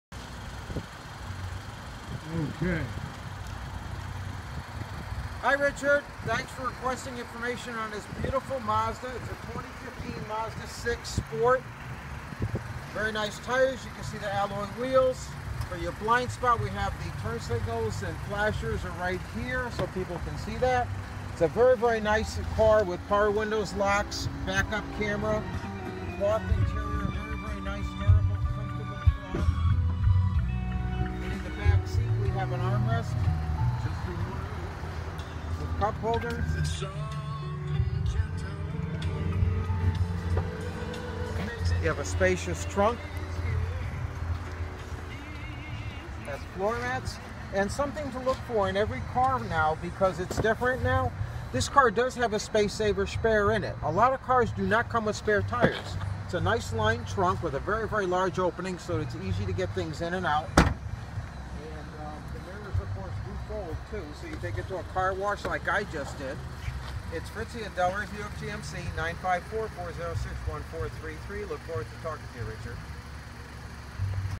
2015 Mazda 6
22,185 miles on this Florida car with a clean CarFax, cloth interior, 2.5 liter/6 speed automatic that returns up to 38 miles per gallon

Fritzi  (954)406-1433